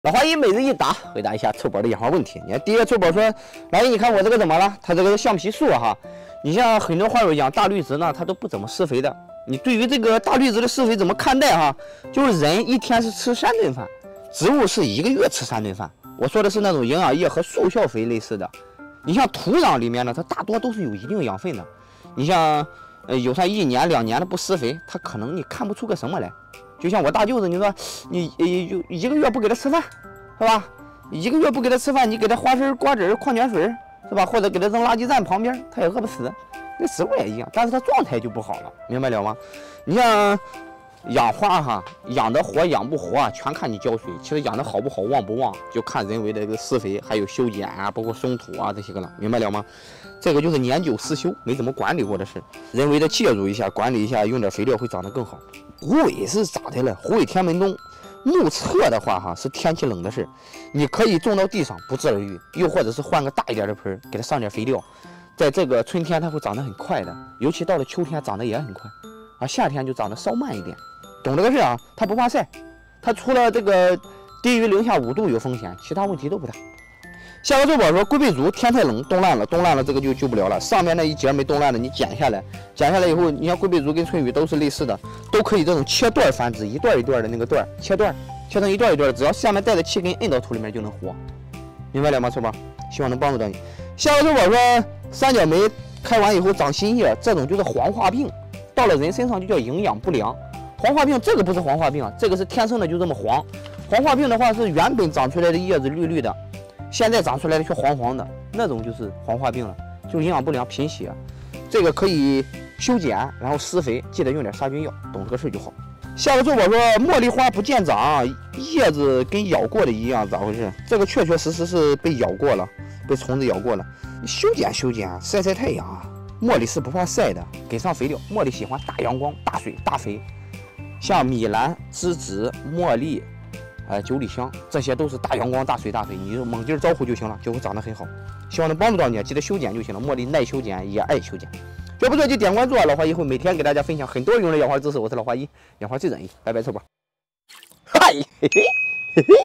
橡皮树耷拉叶怎么回事？三角梅黄叶怎么办？/What's the deal with rubber tree drooping leaves?
今日提问：橡皮树、狐尾天门冬、龟背竹、三角梅、茉莉/Today's question: Rubber tree, foxtail asparagus, monstera, triangular plum, jasmine